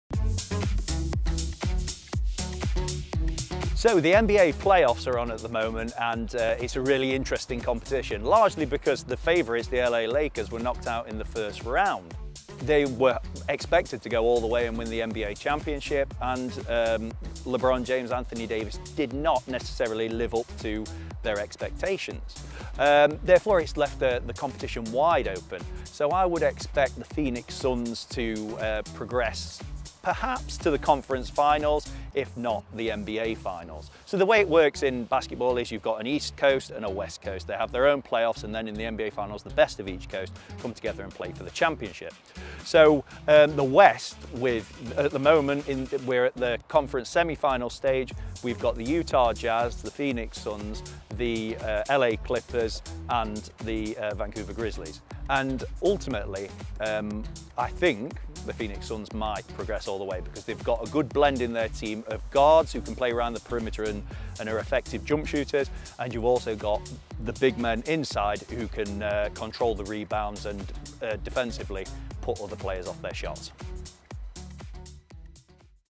NBA playoffs - who will be victorious?
After the pandemic put a stop to major sporting events in 2020, the world is ready to celebrate a bumper summer of sport and our StaffsSquad are here to bring you expert  insights every step of the way.

After favourites the LA Lakers were knocked out in the first round, the NBA playoffs have been blown wide open. So who will be victorious? Sports coaching expert Ashley Gill gives us his predictions.